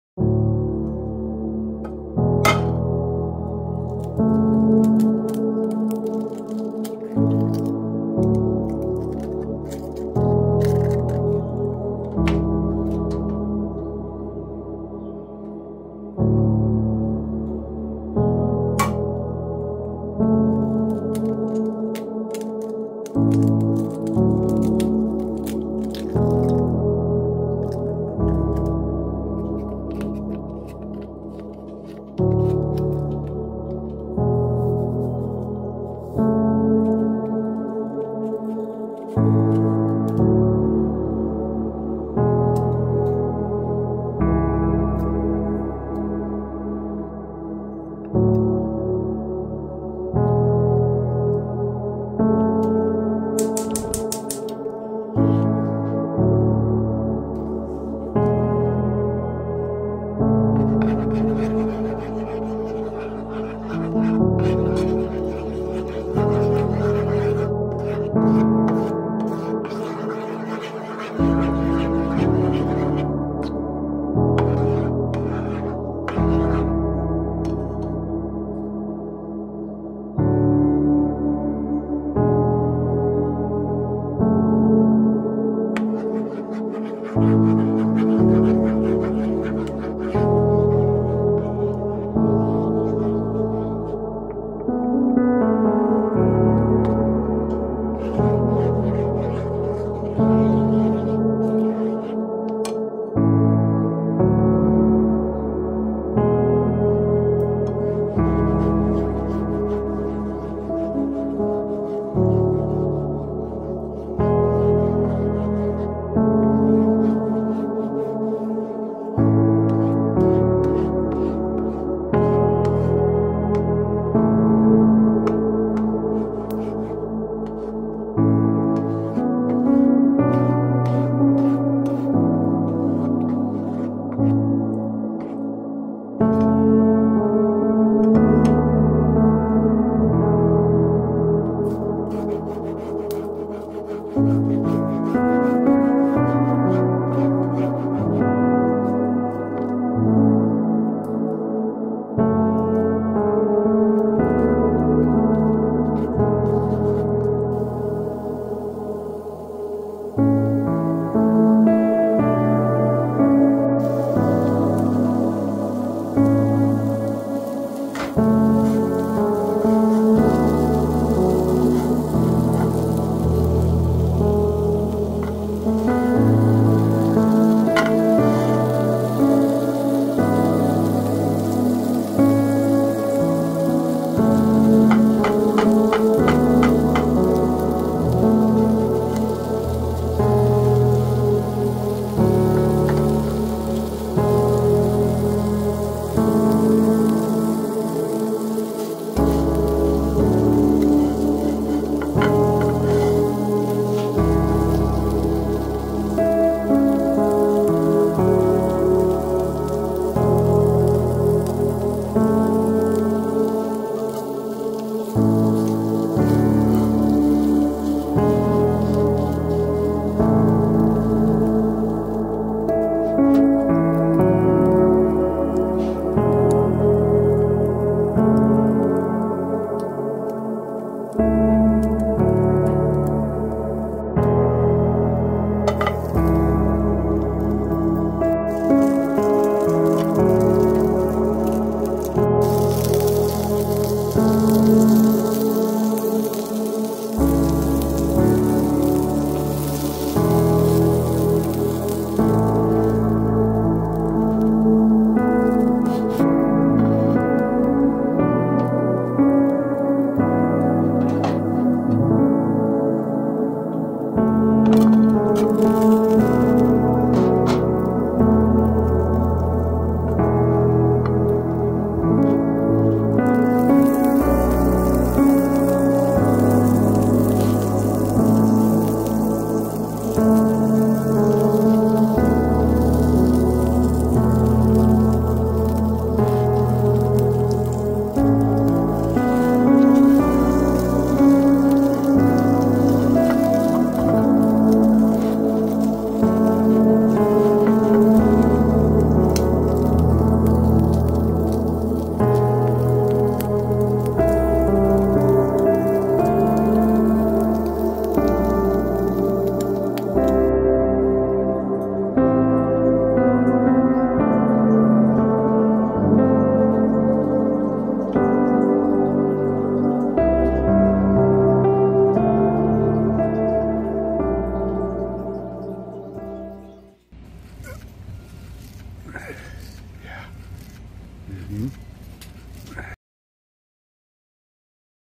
How to make a double egg muffin.
2 eggs. Giant chunk of cheddar. Onions. Giardeniera. Garlic salt. Black pepper. Sausage. Muffin.